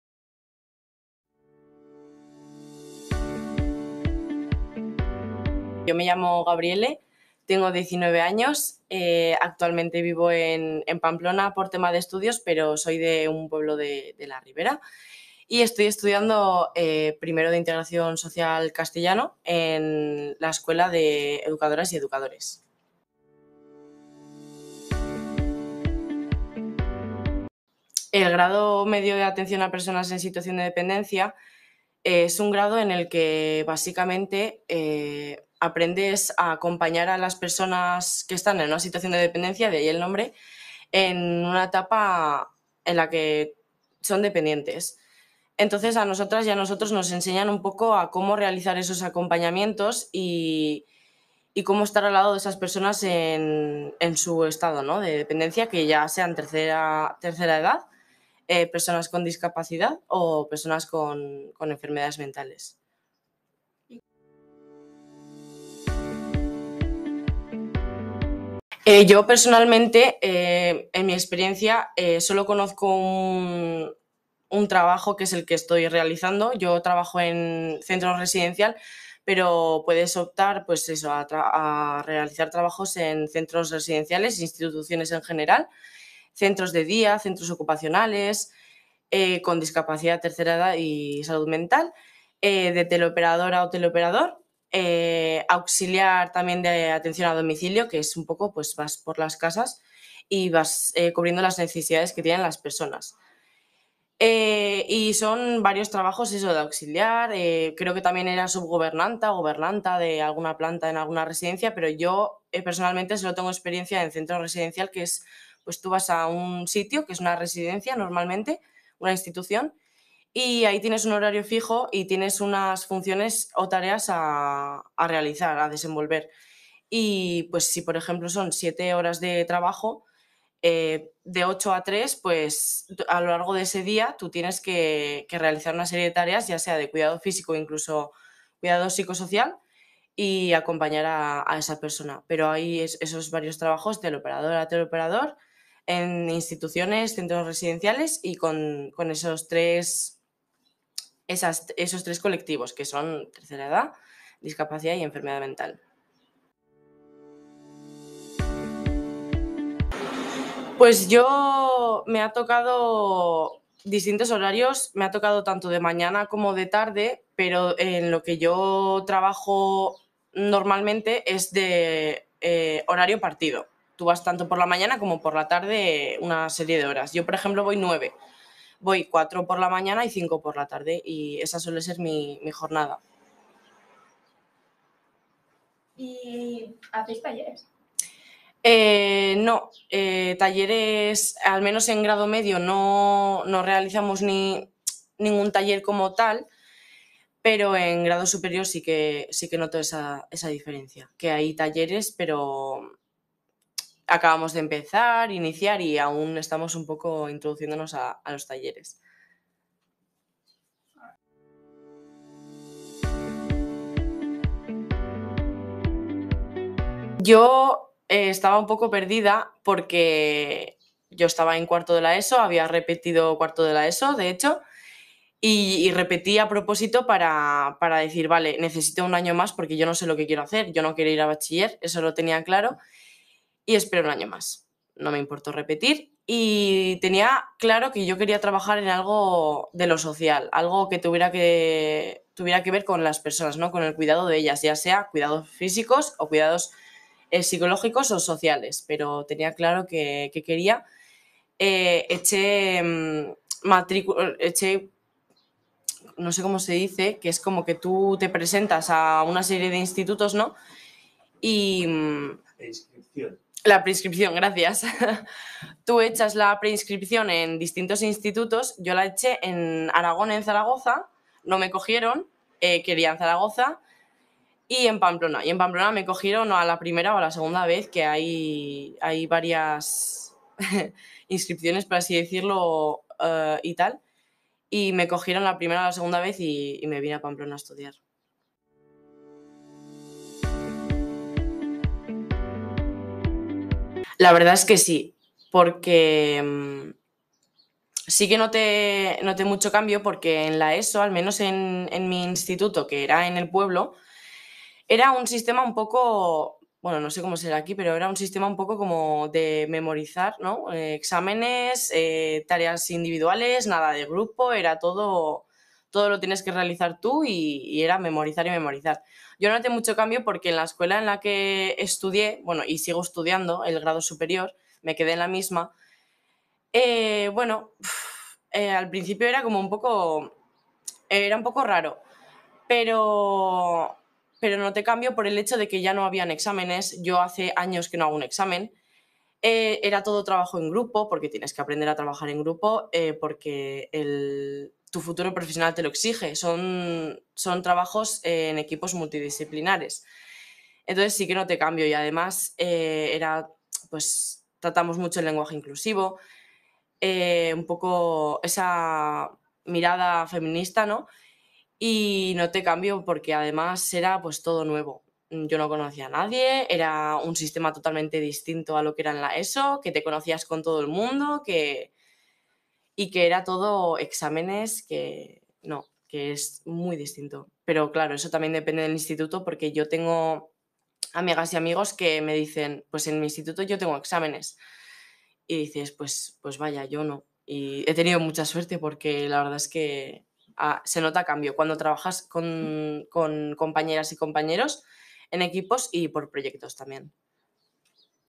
Atención a personas en situación de dependencia
Entrevista a Gabriele Matroxka sobre el ciclo de grado medio "Atencion a personas en situacion de dependencia" que pertenece a la familia profesional "Servicios socioculturales y la comunidad". Realizado por el alumnado del IES Julio Caro Baroja de Pamplona, en el proyecto "Elige tu futuro"